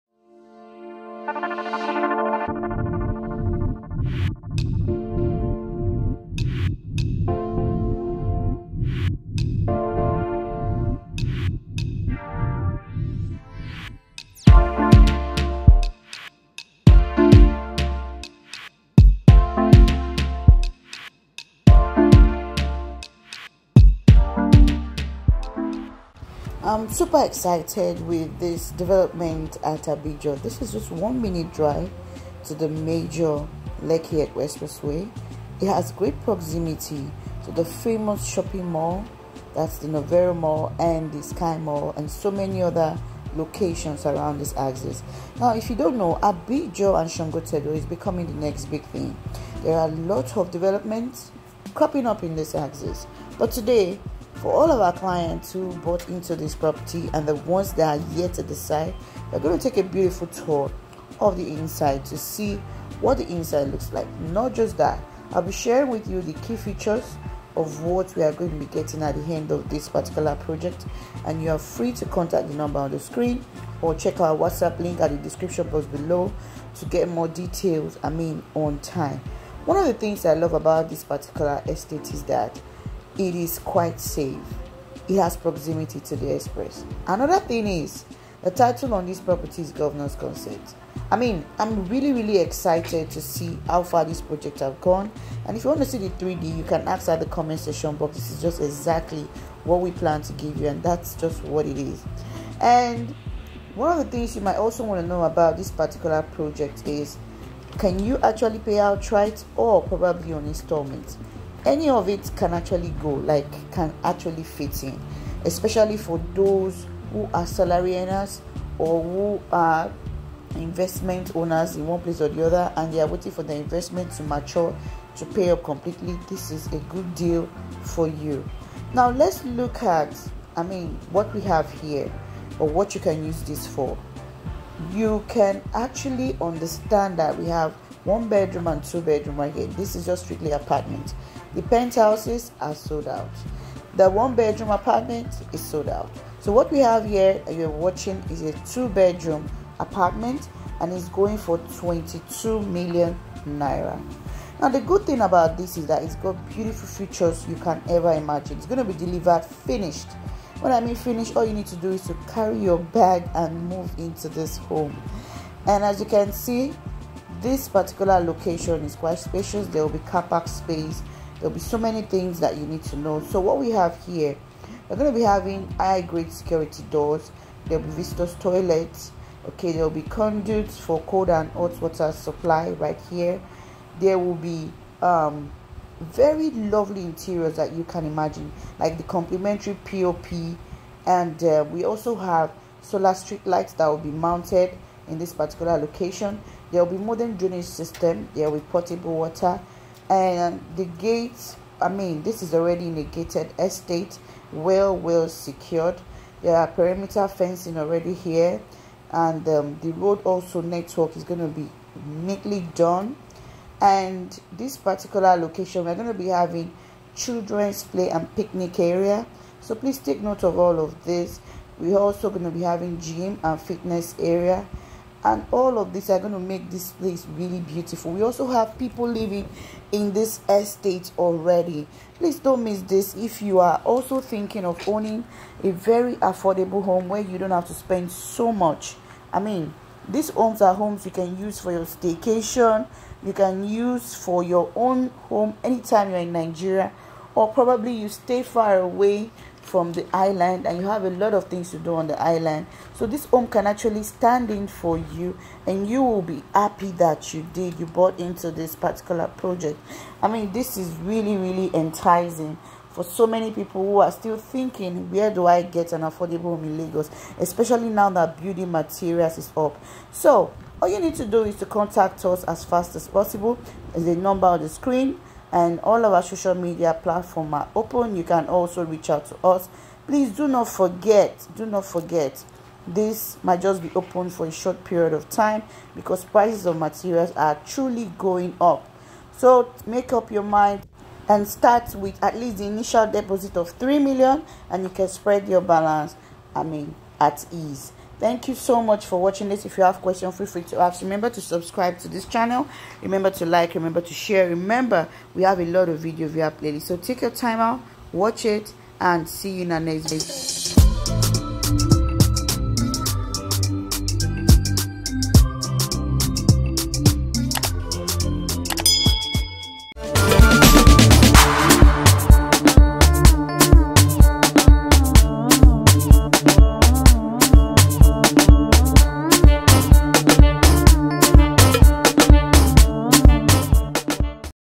Inside ₦22m [$32, 000] Tastefully Finished Apartment To Own At Abijo Lekki Lagos Nigeria
Land Available  Here: 648sqm | ₦20m inclusive of other statutory fees.

₦17m  1 Bedroom Apartment  - Sold Out
₦22m 2 Bedroom Apartment
₦40m 3 Bedroom Terrace

Title is Governor's Consent

Deposit N3m and spread balance to 6 & 12  months.

PAY Outright for 2 units & WIN FREE Plot Of Land In Lagos.

Key Features:
* All Rooms Ensuite
*High grade security doors.
* Visitor's Toilet
*Conduit cold and hot water supply piping
* Free 3.5KVA Inverter for outright purchase.
* Fully Completed Exterior
*Contemporary POP ceiling finish.
* Solar Street Light
* Modern Drainage System
* Potable water
* Gated and interlocked compound
* Perimeter Fencing
* Impeccable road network.
*Designated parking.
*Amazing land scape open spaces.
*Children's play/picnic area.
*Gym and fitness area

Neighborhood Attraction:
*Novare Mall Lekki
*Lagos Business School
*Corona School Lekki

Watch | Like | Comment | Tag | Share :
For further enquiries & Inspection & Real Estate Advice: